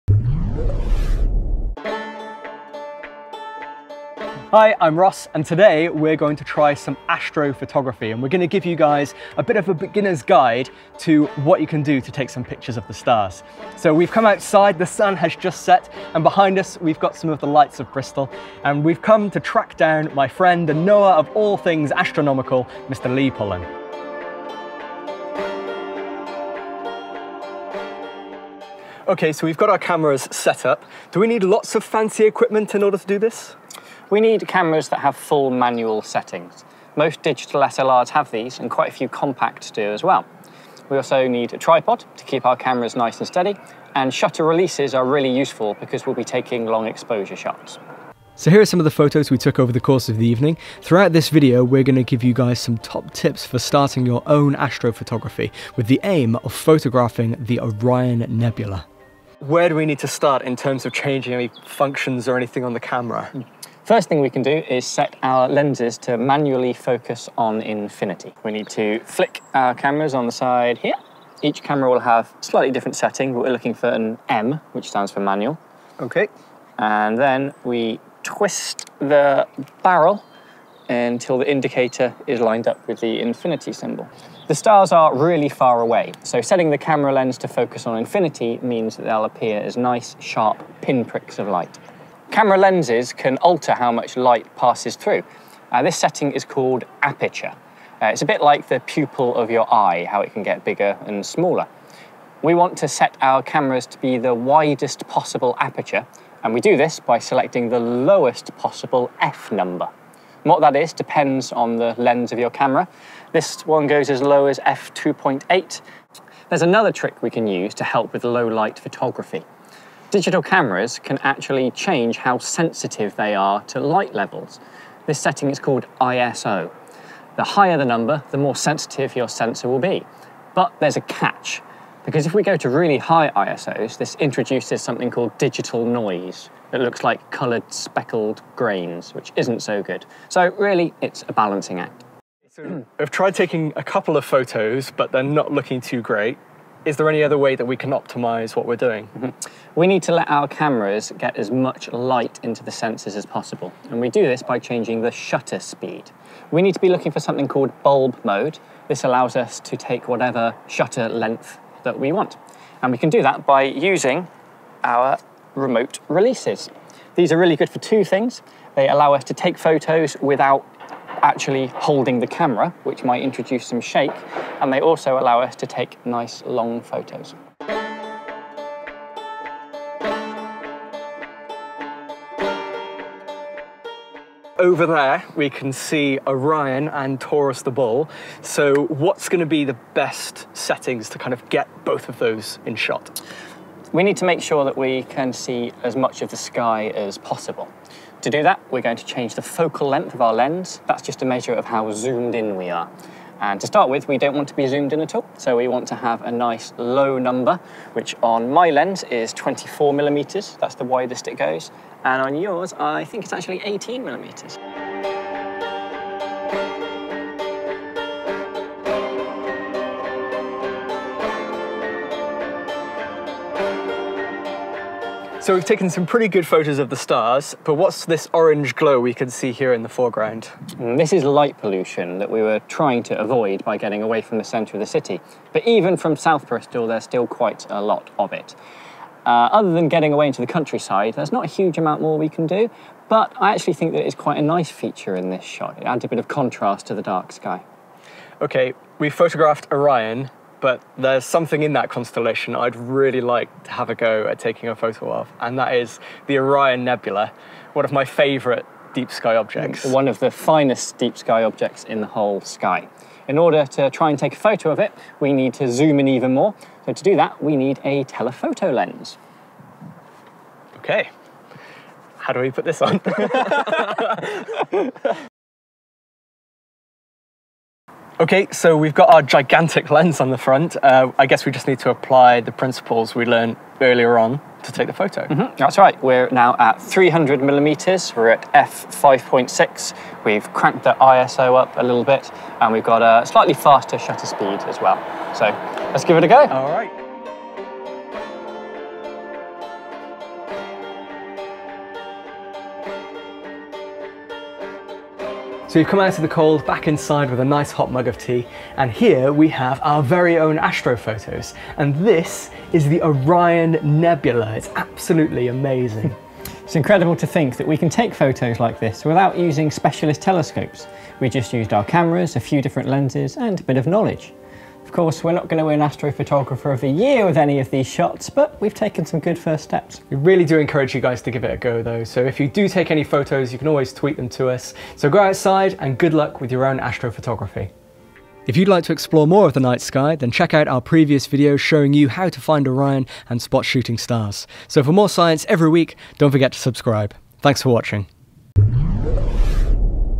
How to photograph the night sky | A Beginner's Guide to Astrophotography | We The Curious
Learn how to take photos of the night sky, constellations, and deep sky objects like the Orion Nebula in this beginner's guide to astrophotography, with Lee and Ross of the Live Science Team.

This video was presented by: Ross Exton & Lee Pullen
Produced by Ross Exton & Seamus Foley

We took our astrophotos using the following settings, but feel free to experiment and let us know how you got on...

STAR TRAILS
Camera: Nikon D700
Lens: Nikon AF-S VR 70-300mm f/4.5-5.6G IF-ED
Focal length: 300mm
Aperture: f5.6
ISO: 200
Shutter speed: 120 seconds

THE MOON
Camera: Nikon D700
Focal length: 300mm
Aperture: f5.6
ISO: 200
Shutter speed: 1/100th second

JUPITER AND MOONS
Camera: Nikon D700
Focal length: 280mm
Aperture: f5.6
ISO: 800
Shutter speed: 1 second

PLEIADES
Camera: Nikon D700
Focal length: 300mm
Aperture: f5.6
ISO: 1250 (high ISO shot was 6400)
Shutter speed: 1.3 seconds

GEMINI AND JUPITER
Camera: Nikon D700
Lens: Nikon AF-S 24-70mm f/2.8 G ED
Focal length: 42mM
Aperture: f2.8
ISO: 400
Shutter speed: 20 seconds

ORION AND TAURUS
Camera: Nikon D700
Focal length: 42mm
Aperture: f2.8
ISO: 200
Shutter speed: 15 seconds

ORION NEBULA
Camera: Nikon D700
Focal length: 155mm
Aperture: f4.5
ISO: 2000
Shutter speed: 2 seconds